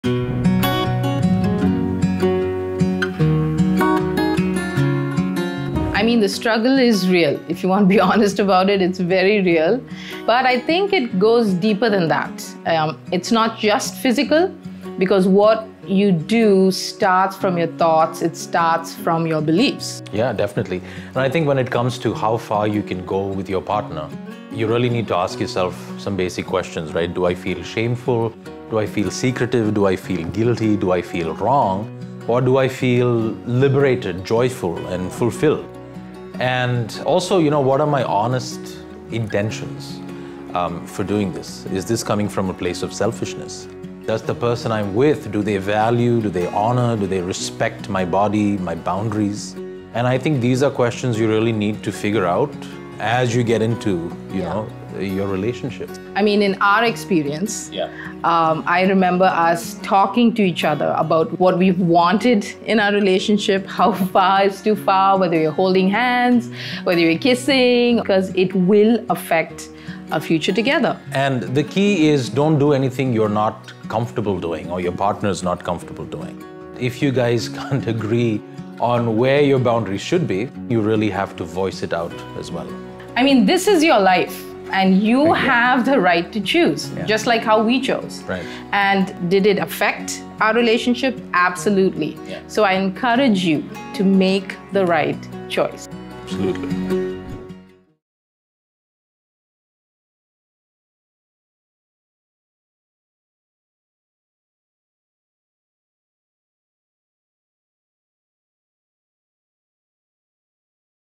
Holding Hands, Kissing, Sex? How far can we go? | Luke & Roshni | Dating Done Right
"How far can I go with my partner?"

When we're in love, we want to get closer to the other person. We want to spend time with them, we want to get to know them better and we want to get a little physical at times as well. So, how far can we go? After all, boundaries are important.

So, can we kiss? Can we hold hands? Can we have sex? Or should we not even touch? Well, it's important for us to think about these things and talk about them with our boyfriend or girlfriend as well. You may not think about it that much, but what you do now, while dating can affect your future as well. So, check out what Luke and Roshni Arulraj have to say.